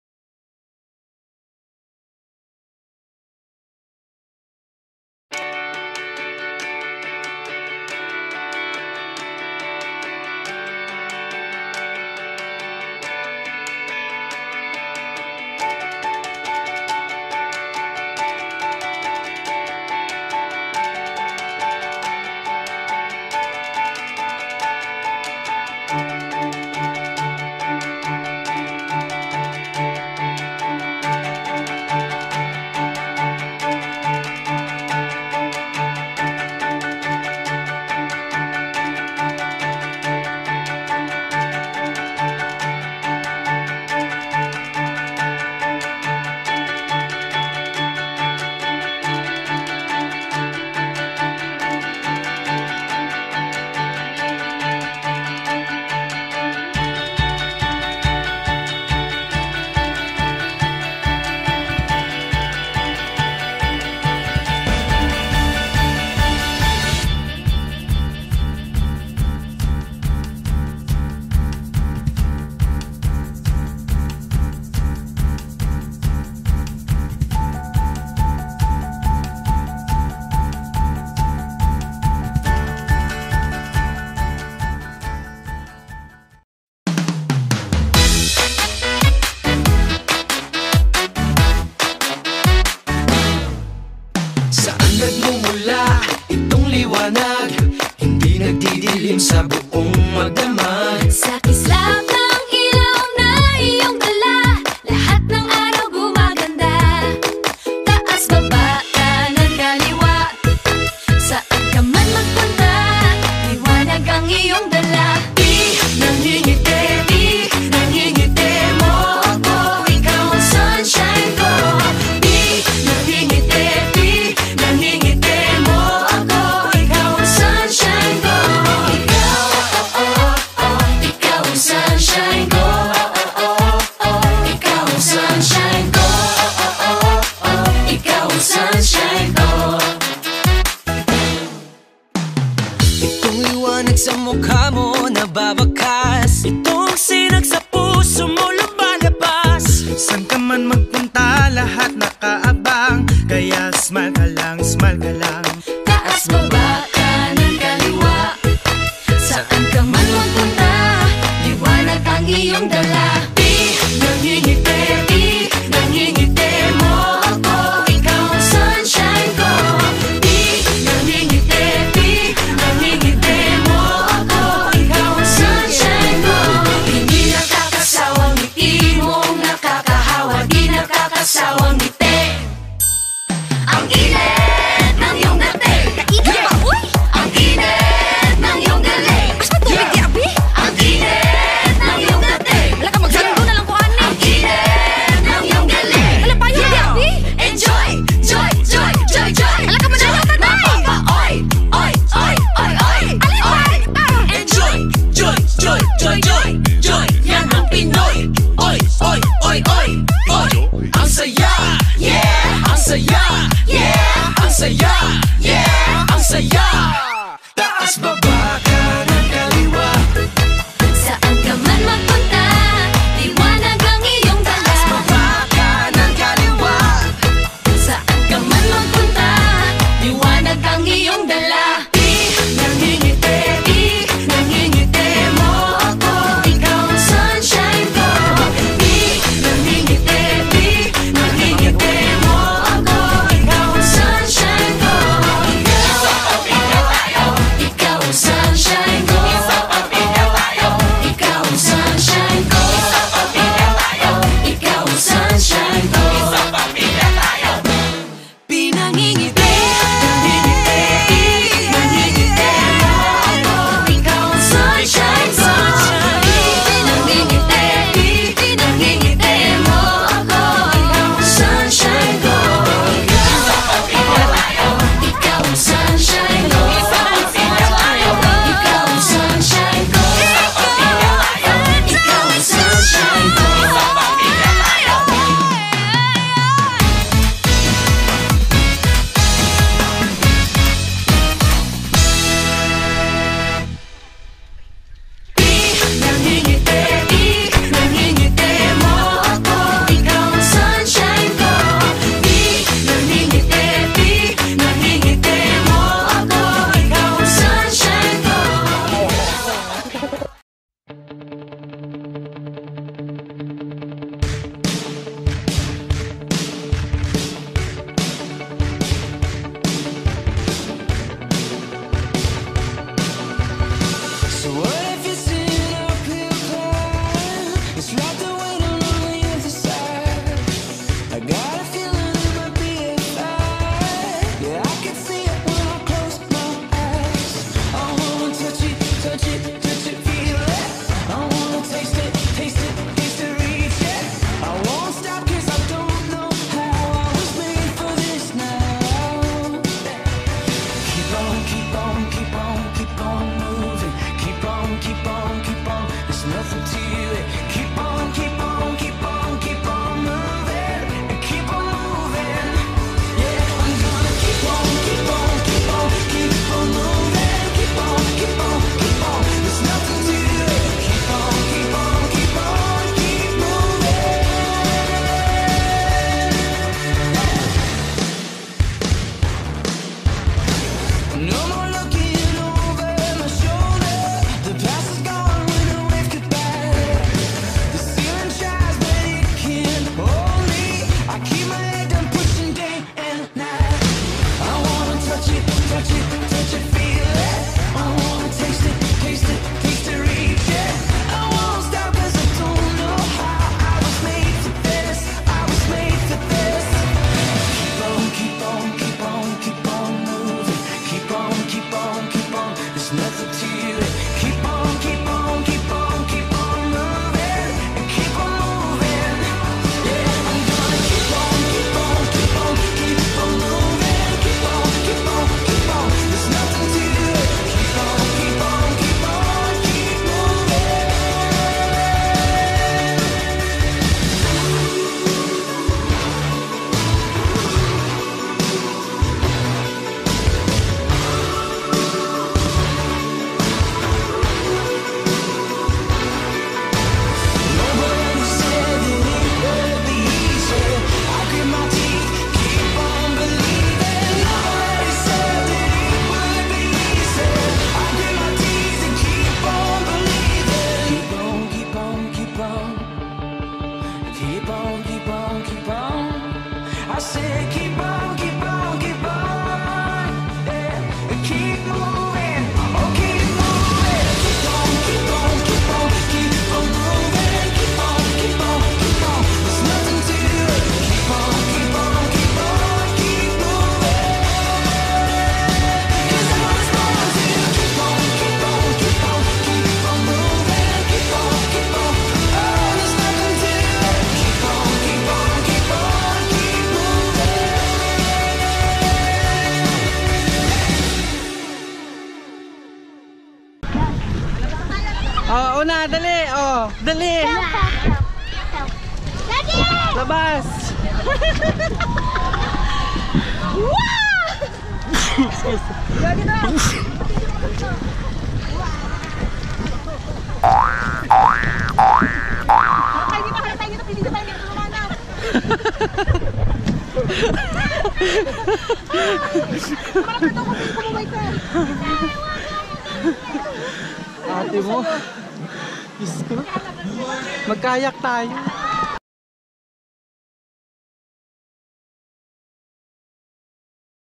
Escala Tagaytay and Club Balai Isabel Batangas
Our May 2017 Lakwatsa at Escala in Tagaytay and Club Balai Isabel in Batangas. Aqua Park is fun!